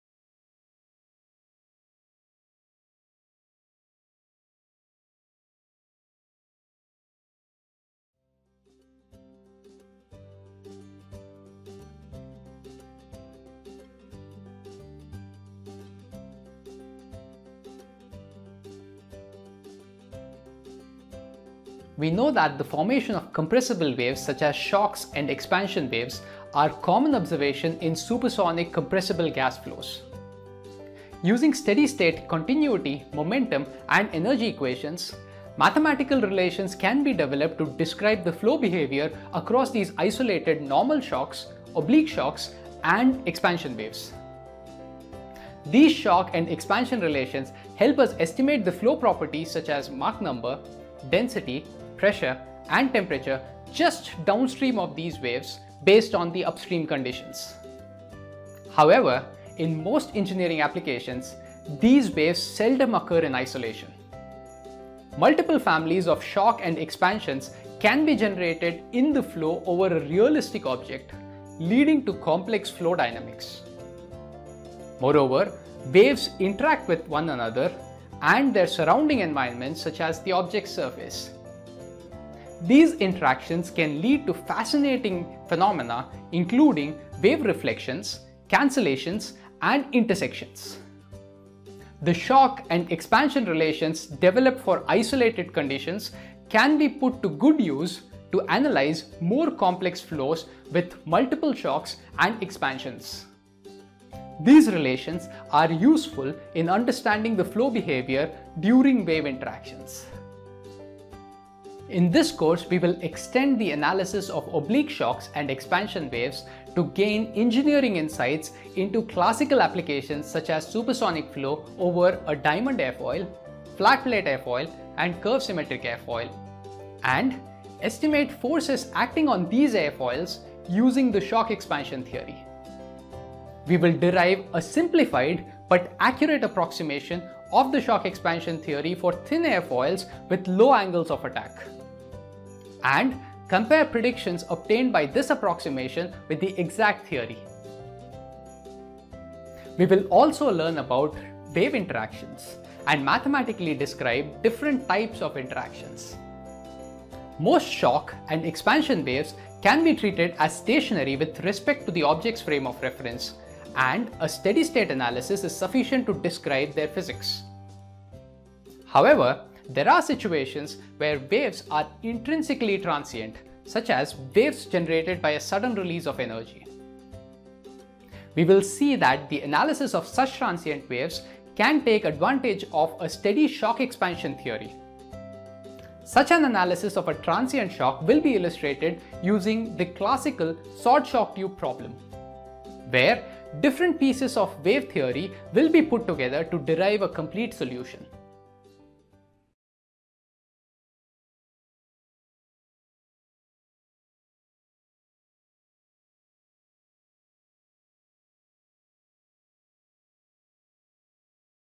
Introduction to Applications of Shock-Expansion Theory — Lesson 1
This video lesson explains that the formation of compressible waves such as normal shocks, oblique shocks and expansion waves is common in supersonic, compressible gas flows. Moreover, they seldom occur in isolation. Instead, they interact with each other and surrounding objects surfaces to reflect, cancel and intersect with one another. This lesson is part of the Ansys Innovation Course: Applications of Shock-Expansion Theory. To access this and all of our free, online courses — featuring additional videos, quizzes and handouts — visit Ansys Innovation Courses